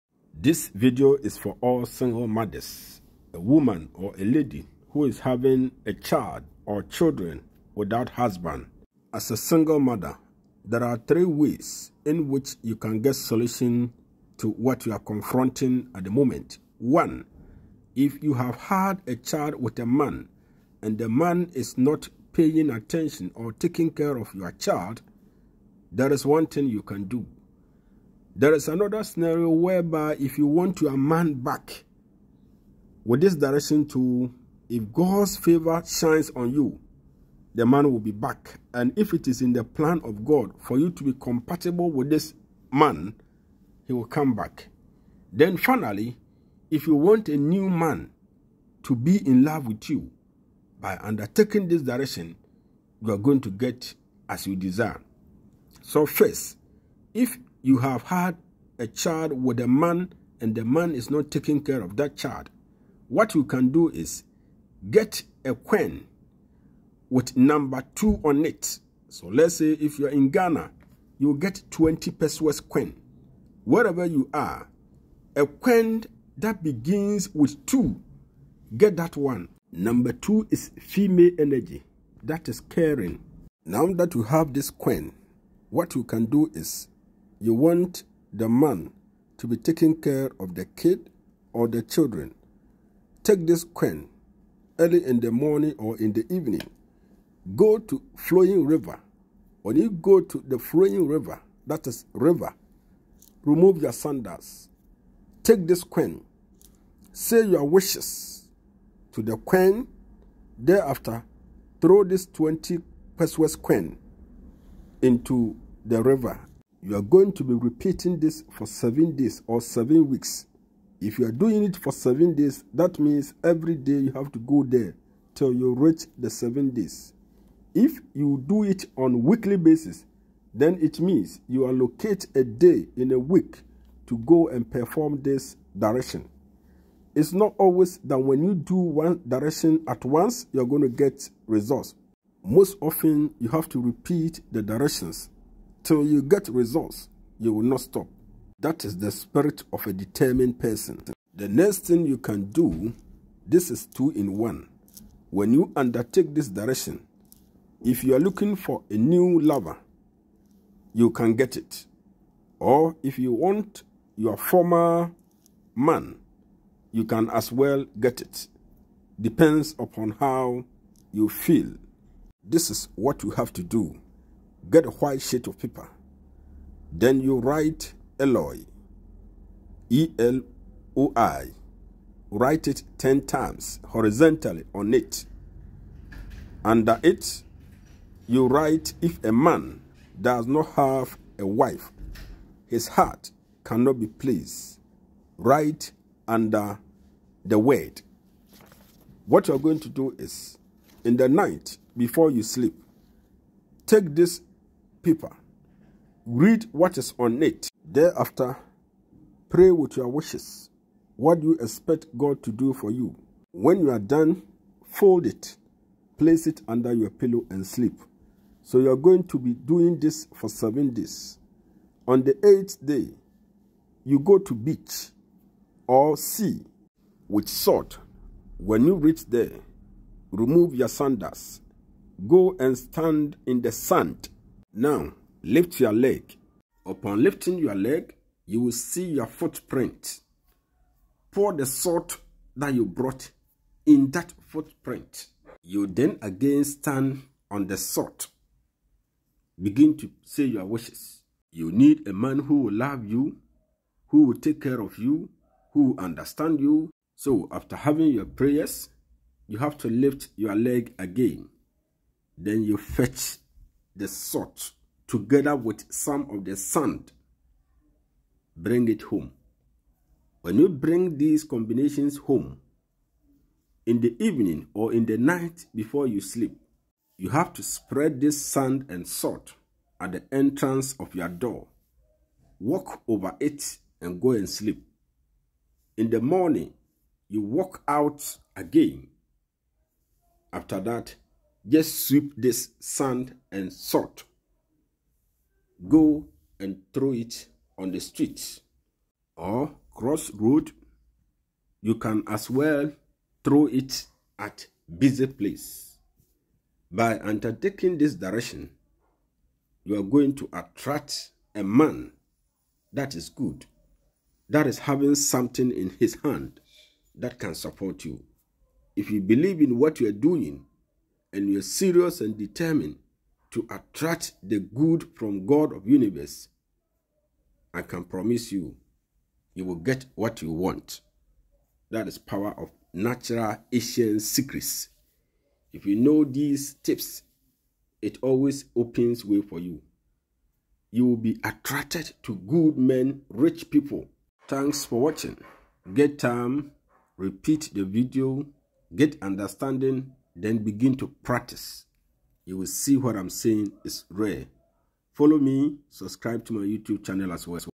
3 Ways For Single Mothers To Find Support, Husbands, And Love.
This video shows you 3 Ways For Single Mothers To Find Support,Husband And Love.